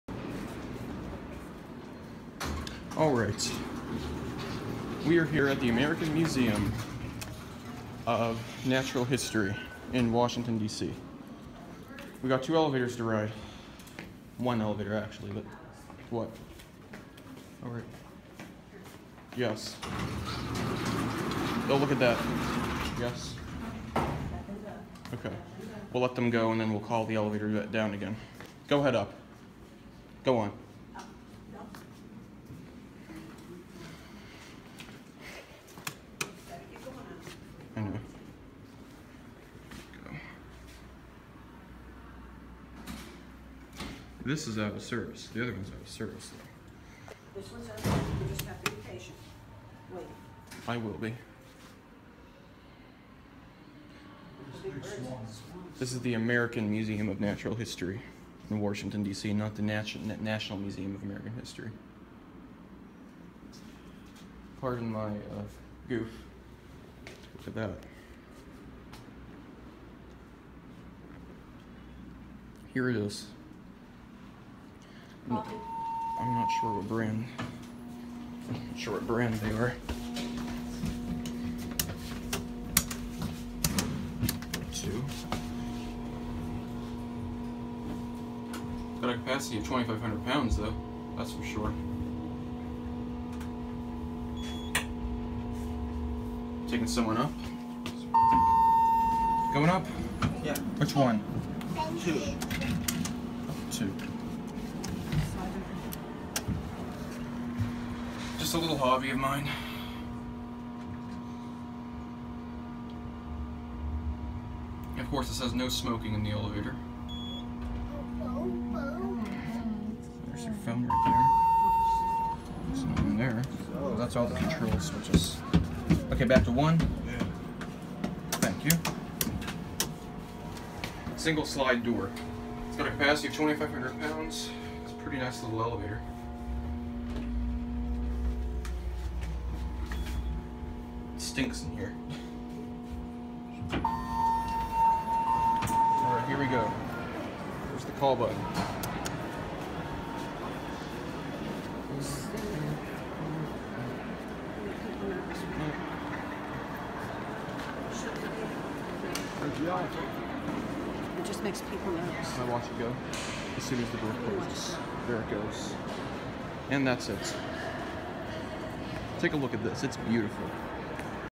Unknown brand elevators @ National museum of natural history in Washington DC
(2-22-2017) These are some very busy elevators but I got a good video for you guys :)